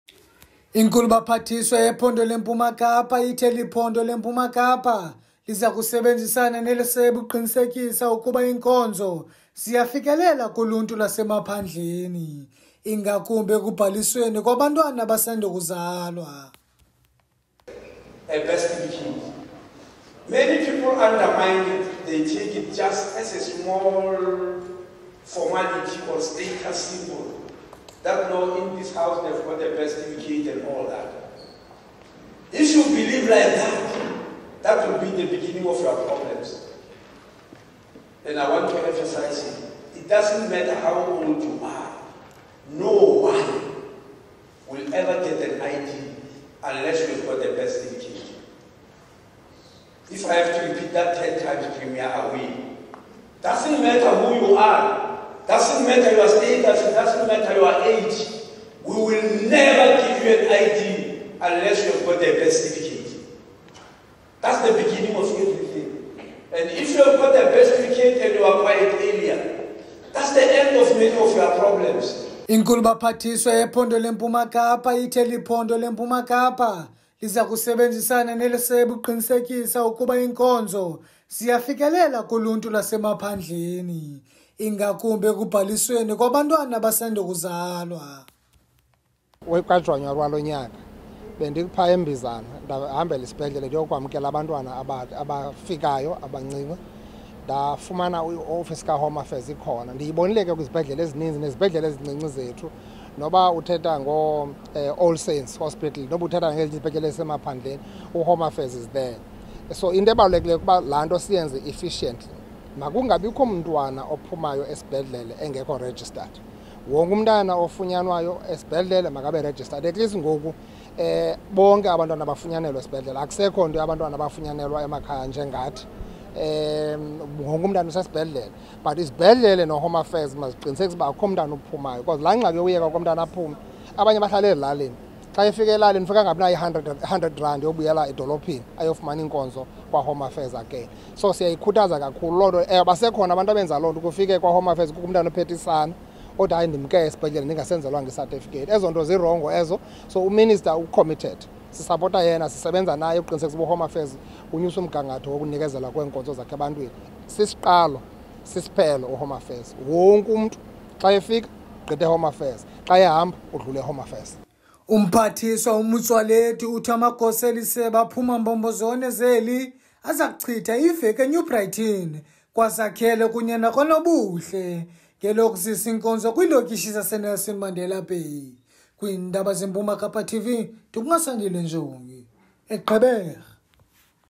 MPK TV NEWS| Isebe lezemicimbhi yekhaya lizisa iinkonzo e-Mpuma Kapa
Urhulumente wephondo le-Mpuma Kapa uthi uzakuqinisekisa ukuba uluntu luyazifumana iinkonzo kwisebe lemicimbi yezekhaya, nanjengoko eliphondo lijongene nengxaki yabantwana abathi bangafumani isibonelelo ngenxa yokungabhaliswa kwelisebe.

U-Mabuyane ukuthethe oku kuthethathethwano nomphathiswa wesebe lemicimbi yezekhaya obelubanjelwe kwi holo I-Nangoza Jebe Hall e-Gqeberha. Intatheli esenzele elinkqaku ngu Asandile Njongi